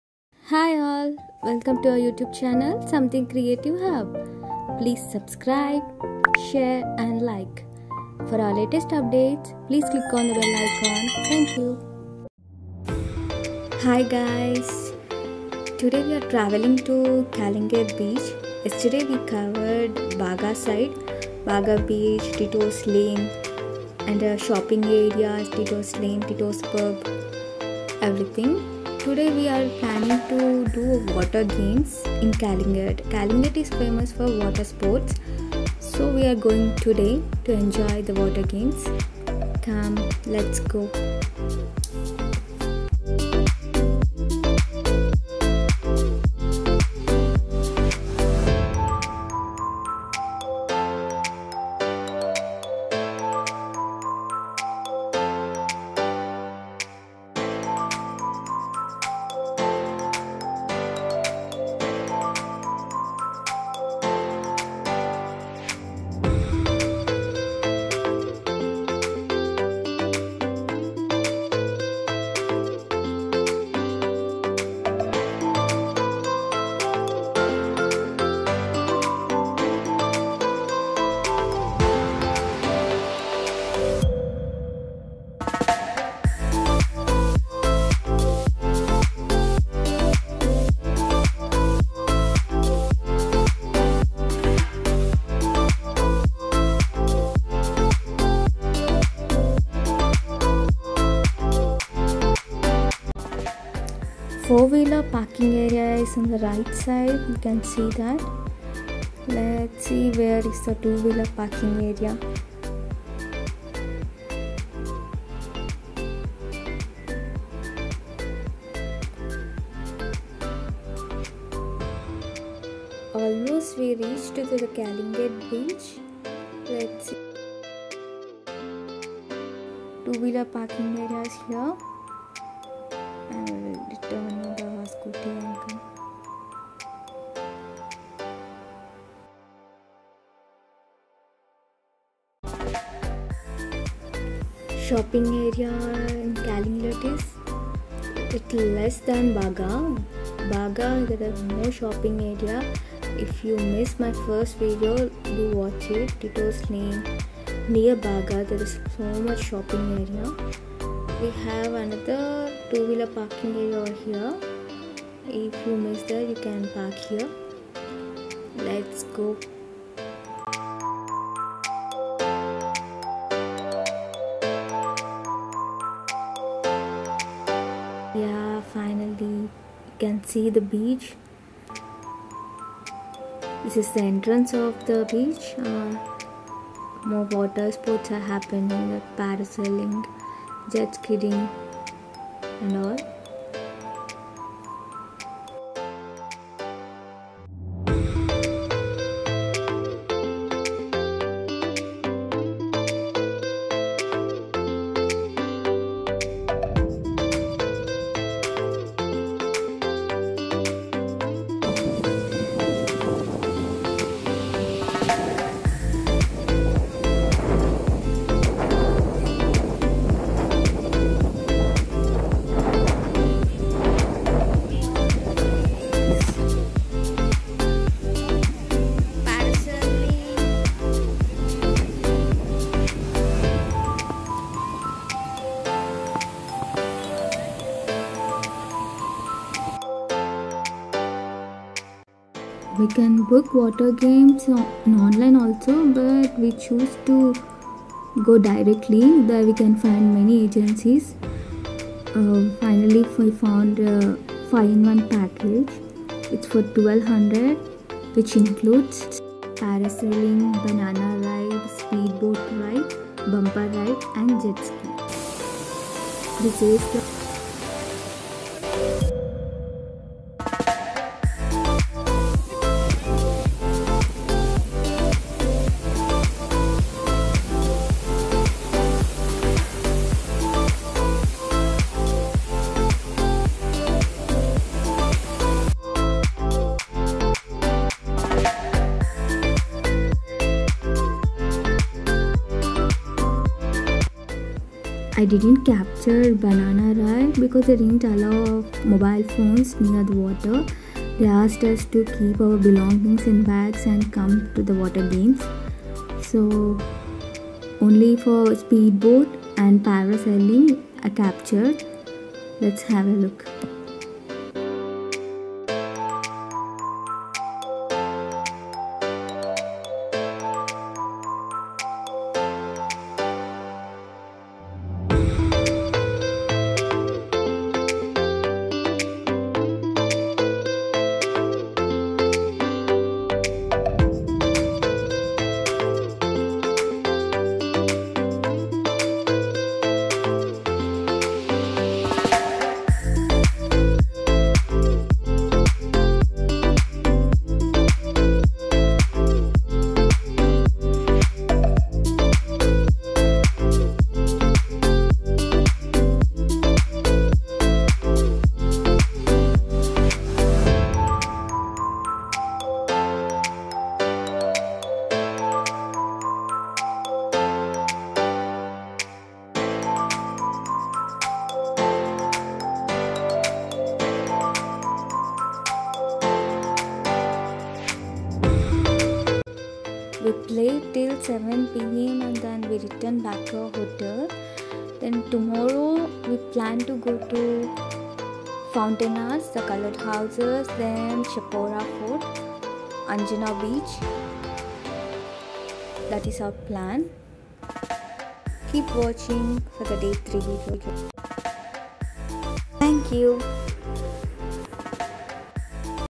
Day 2 in Goa | Calangute Beach | Water sports | Baga - Calangute Beach
Day : 2 in Goa    Calangute Beach

Water Sports : 1200 package
1. Banana Ride
2. Jetski
3.Bumper ride
4. Paraselling
5. Speed Boat

calangute beach
water sports
goa tour after lockdown
beach
goa trip
goa trip in Budget